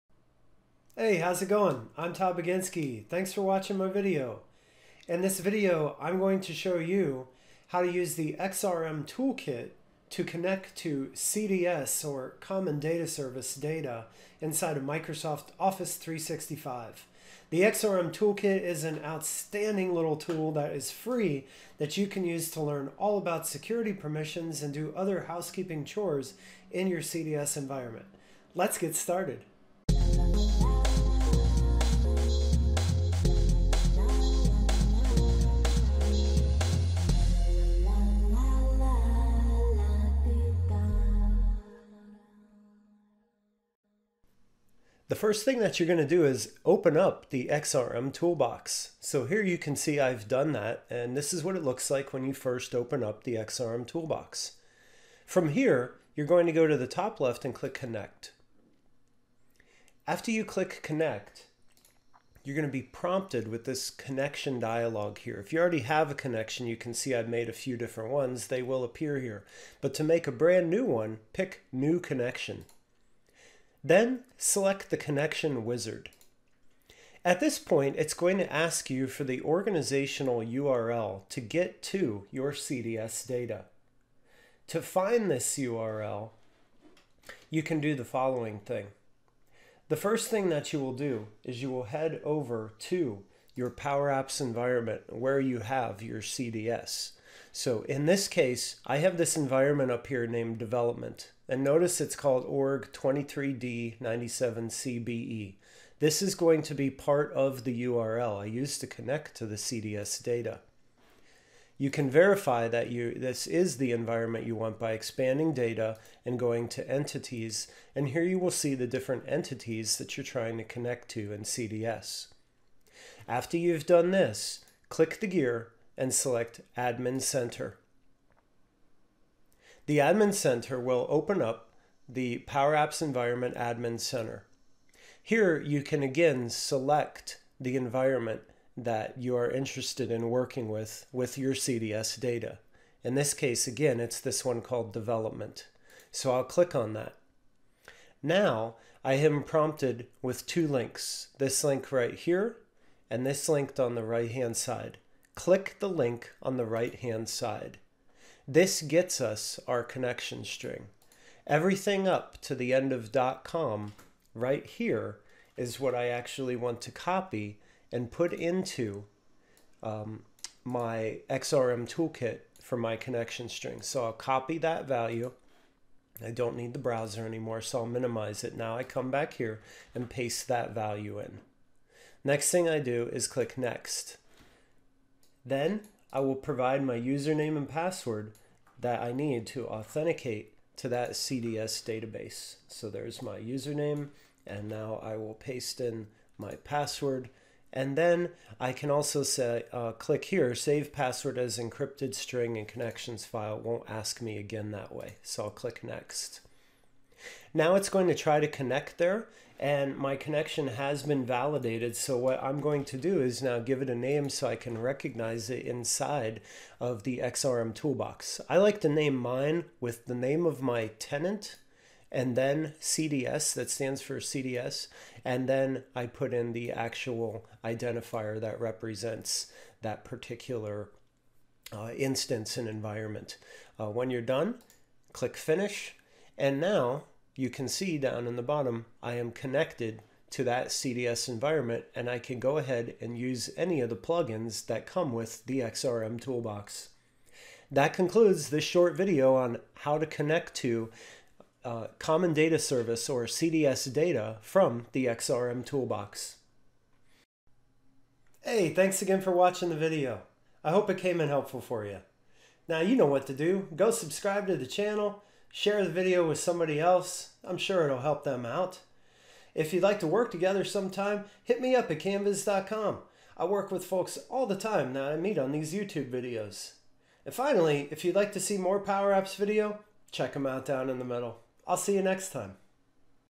Connect XrmToolBox To CDS
In this video I demonstrate how to connect to Common Data Service (CDS) data from the XrmToolBox.

If you need help, consulting, or assistance please reach out